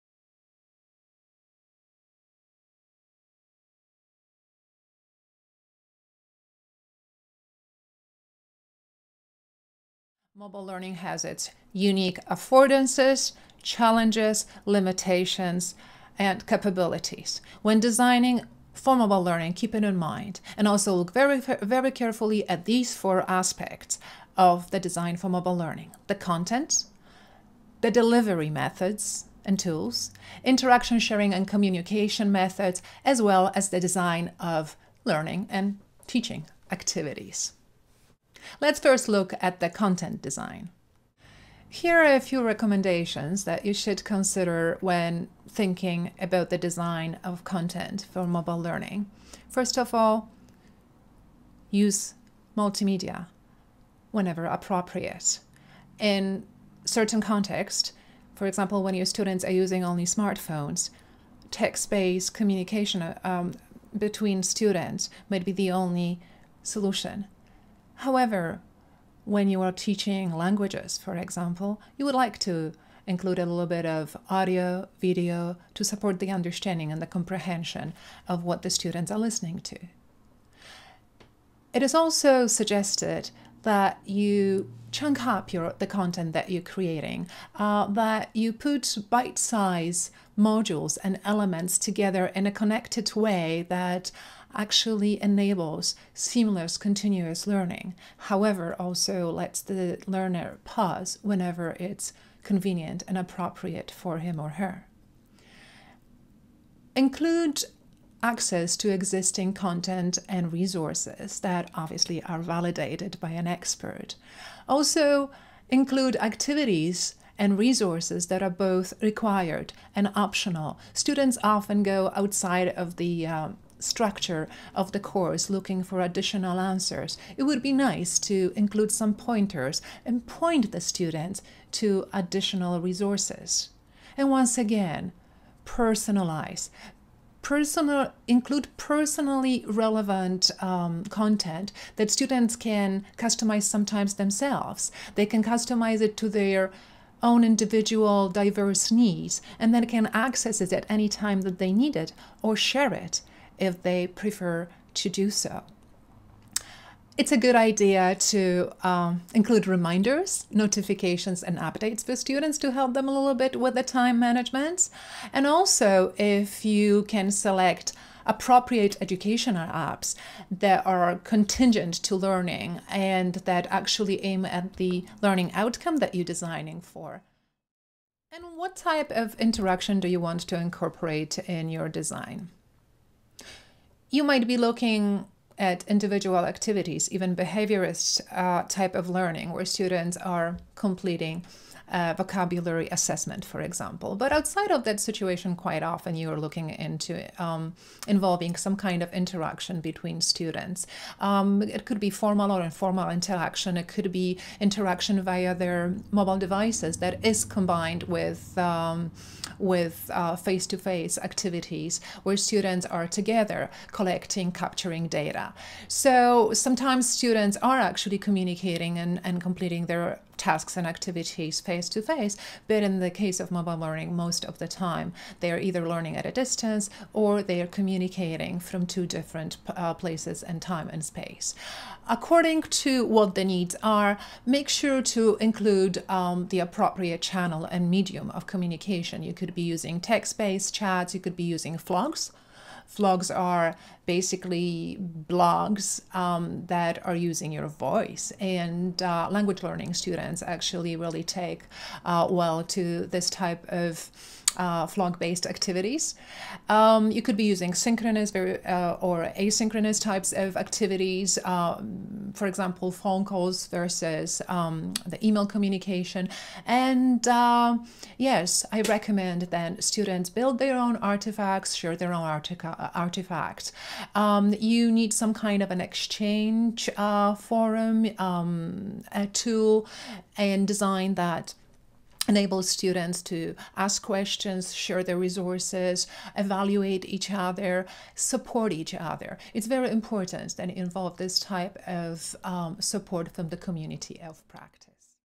Aga Palalas on mLearning Design Considerations
IAmLearn's Dr. Aga Palalas discusses some key considerations for content and interaction elements when designing for mobile learning. This video was compiled using segments excerpted from Dr. Palalas' presentation on Design Principles for Mobile Learning for the MOOC on Mobiles for Development, published under Creative Commons attribution by the Commonwealth of Learning